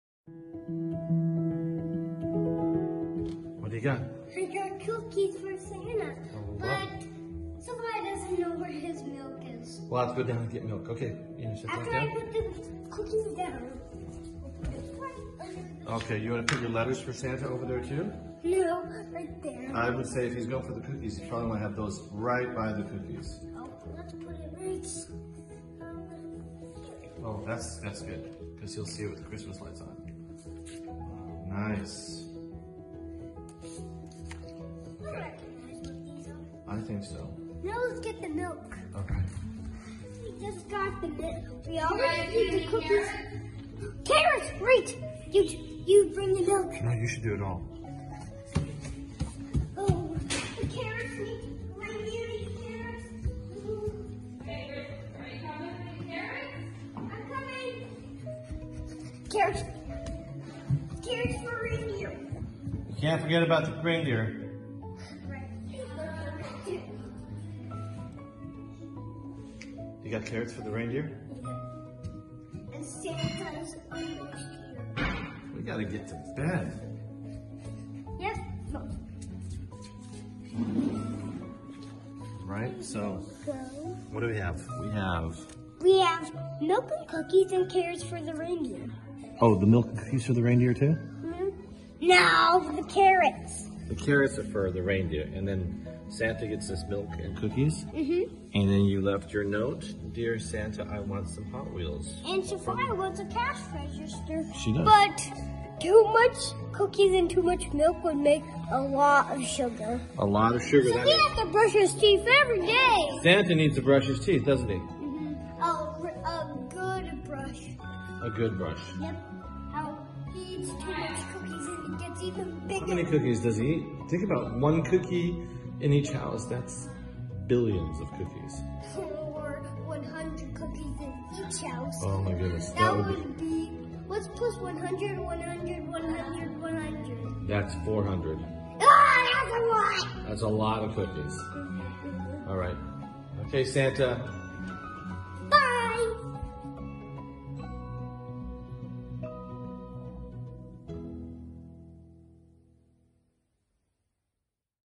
GETTING READY FOR SANTA!!! MILK, COOKIES, and CARROTS... Carrots are for the reindeer, of course!
Time to get to bed, but before we do, we need to get our snack ready for Santa!  Wishing you all a Happy Holidays!